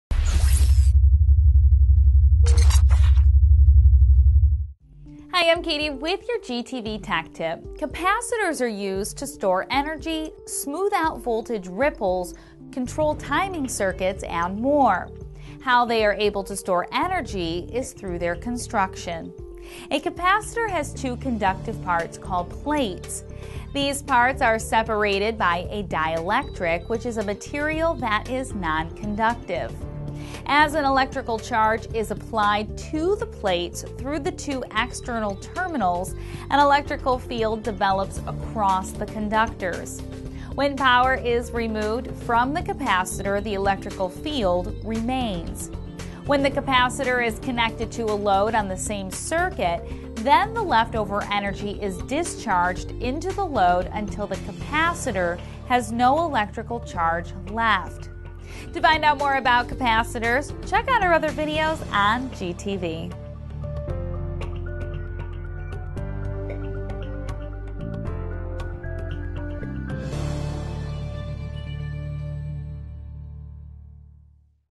How Capacitors Store Energy - A GalcoTV Tech Tip | Galco
How a capacitor works presented by Galco TV.

Check out this video to find out how a capacitor works and how it stores energy.  This video also outlines the many uses capacitors can have in everyday electronics.